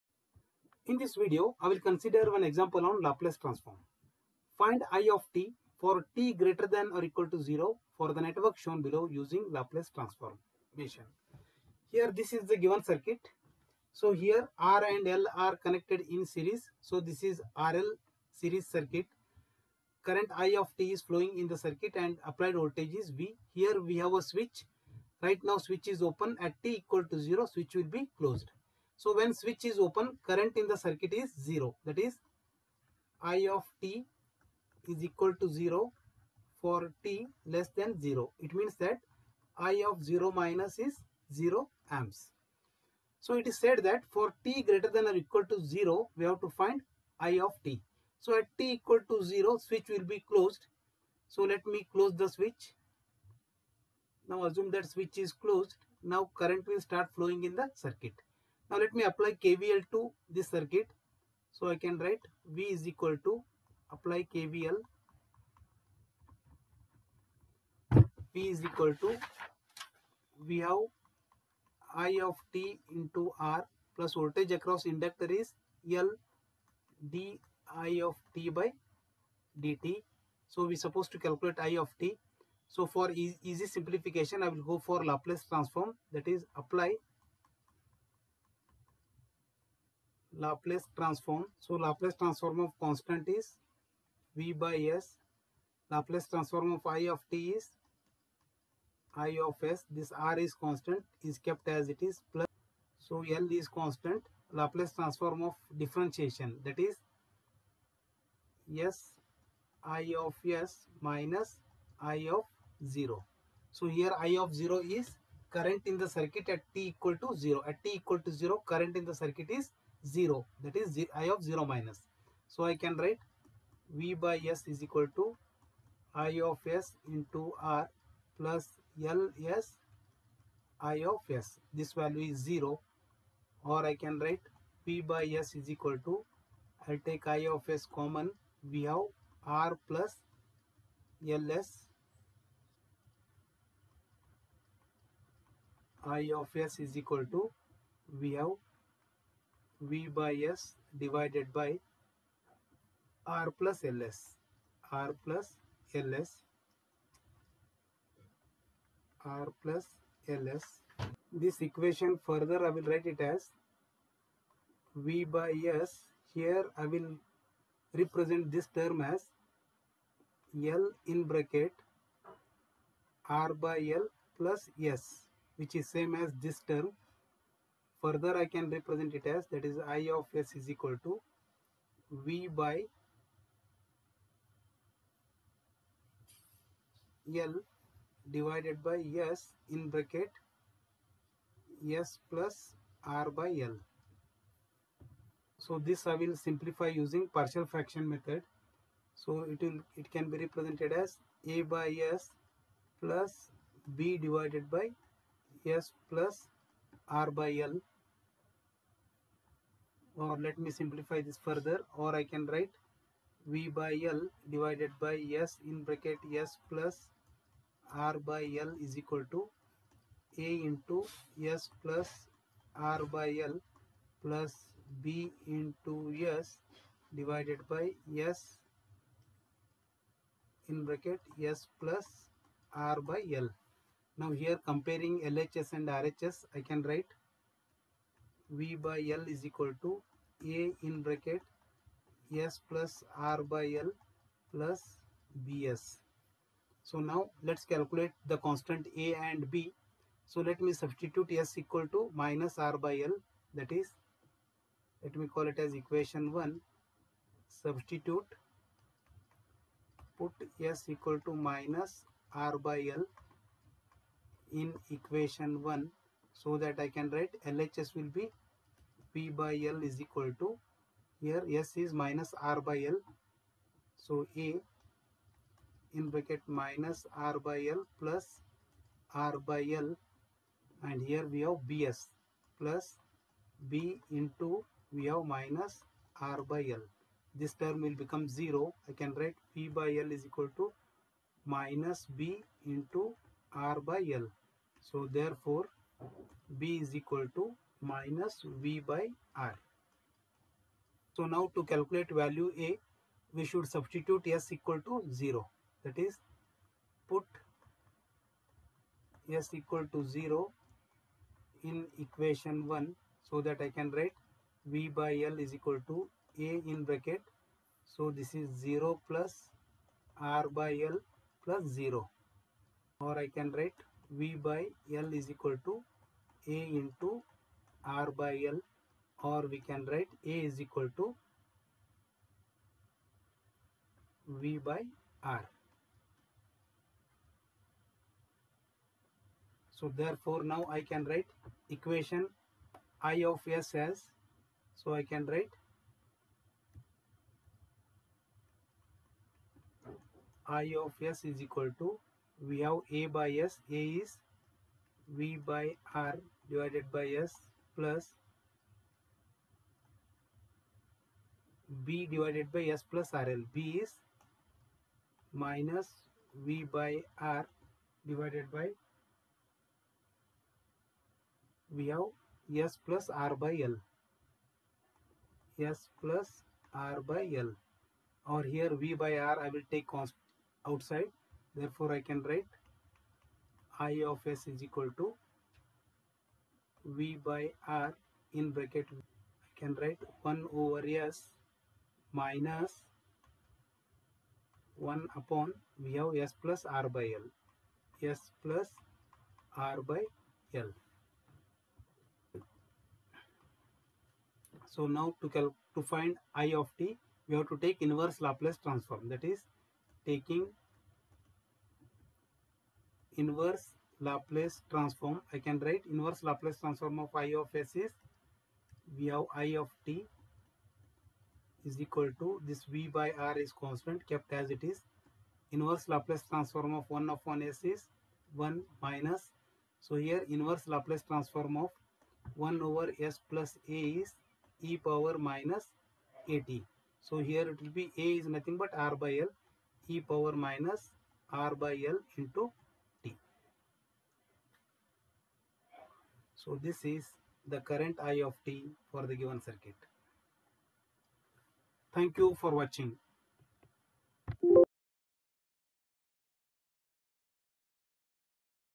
Find i(t) for t≥0, for the network shown below, using Laplace Transformation
Jan 2024 QP -18EE32